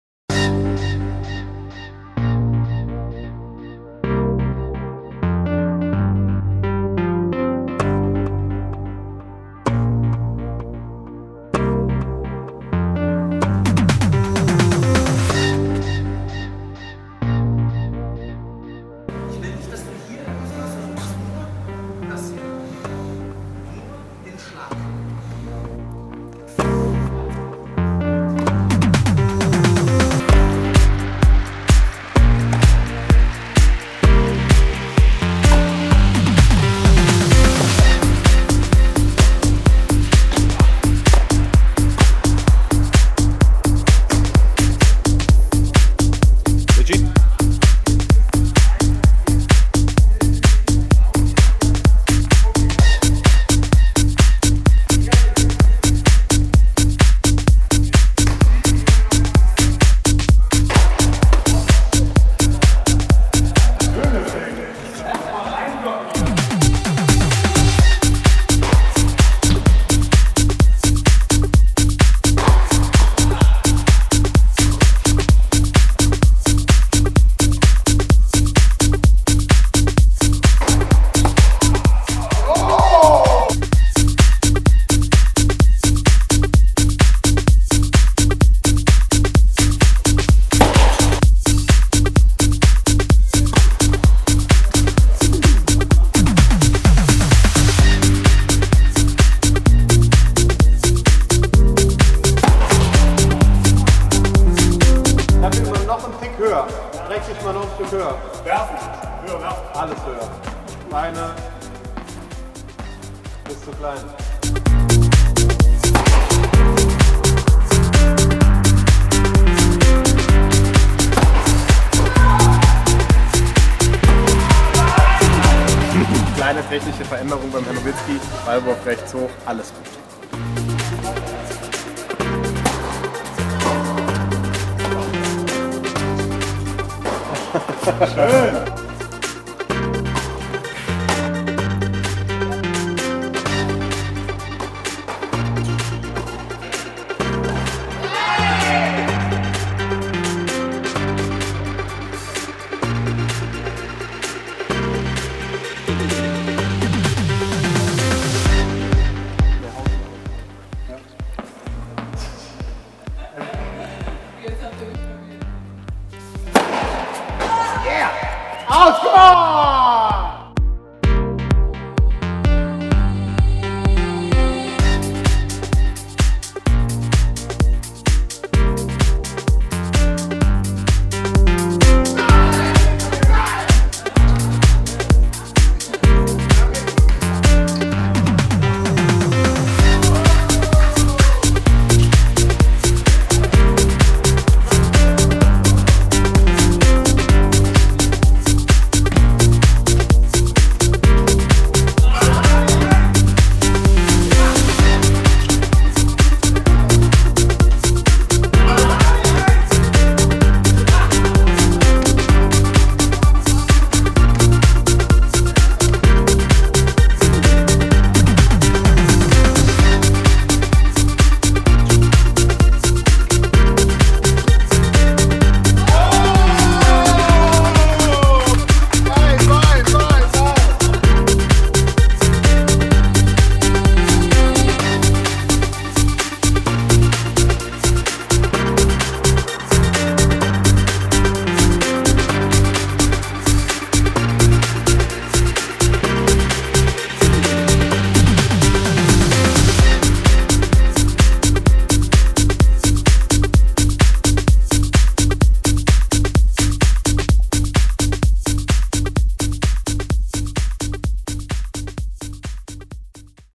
Dirk Nowitzki vs. Andrea Petkovic in der Tennis-University
DIRK NOWITZKI VS. ANDREA PETKOVIC -
oder einer der größten deutschen Sportler besucht die Tennis-University. Schaut selbst, wie sich Dirk vom Trainerteam der Tennis-University coachen lässt, mit Andrea Petkovic Punkte spielt und vieles mehr.